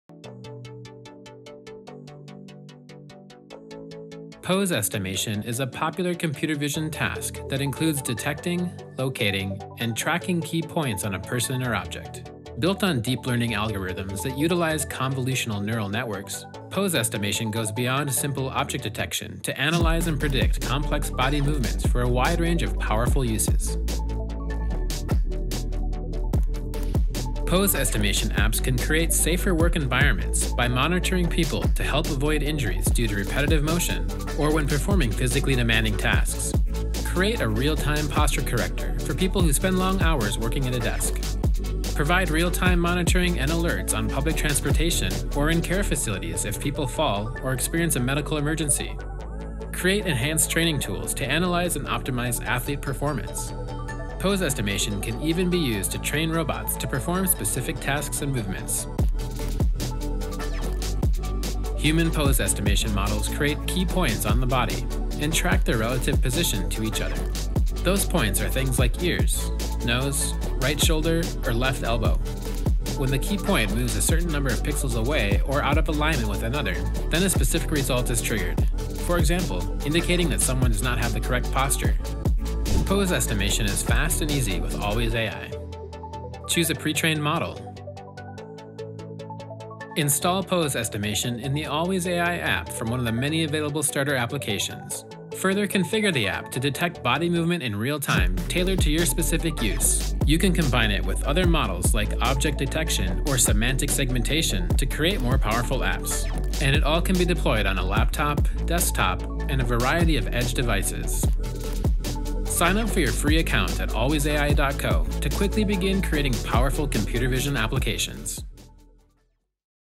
Business Applications for Computer Vision: Pose Estimation
Pose estimation is a popular computer vision task that involves detecting, locating, and tracking key points on a person or object. Pose estimation analyzes and predicts complex body movements for a wide range of uses. In this video you will learn popular use cases for pose estimation, how it works, and how alwaysAI makes leveraging this powerful technology easy!